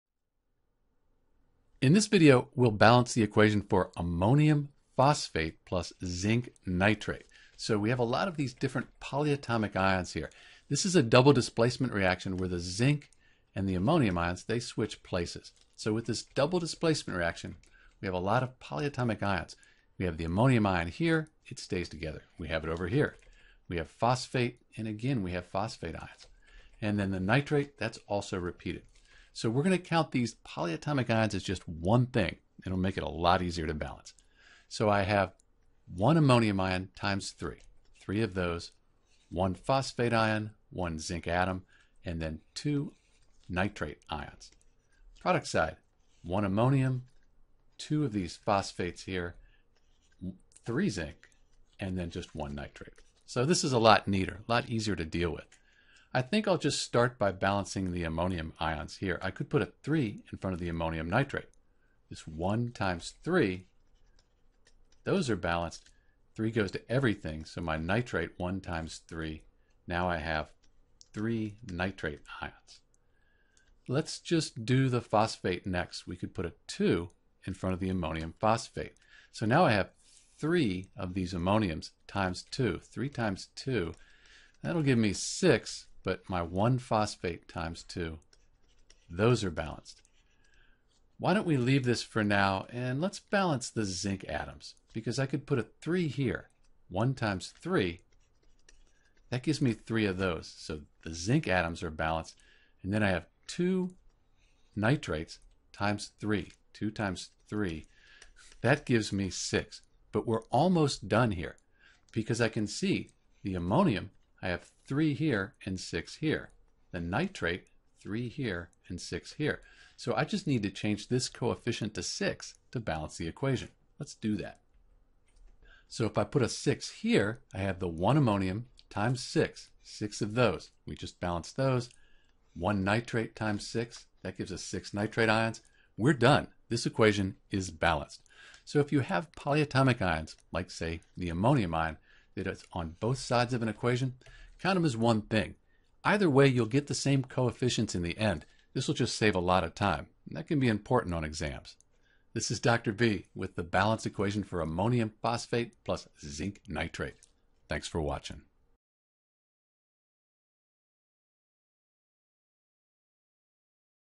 How to Balance (NH4)3PO4 + Zn(NO3)2 = NH4NO3 + Zn3(PO4)2
In this video we'll balance the equation (NH4)3PO4 + Zn(NO3)2 = NH4NO3 + Zn3(PO4)2 and provide the correct coefficients for each compound.

To balance (NH4)3PO4 + Zn(NO3)2 = NH4NO3 + Zn3(PO4)2 you'll need to be sure to count all of atoms on each side of the chemical equation.

Once you know how many of each type of atom you can only change the coefficients (the numbers in front of atoms or compounds) to balance the equation for Ammonium phosphate + Zinc Nitrate.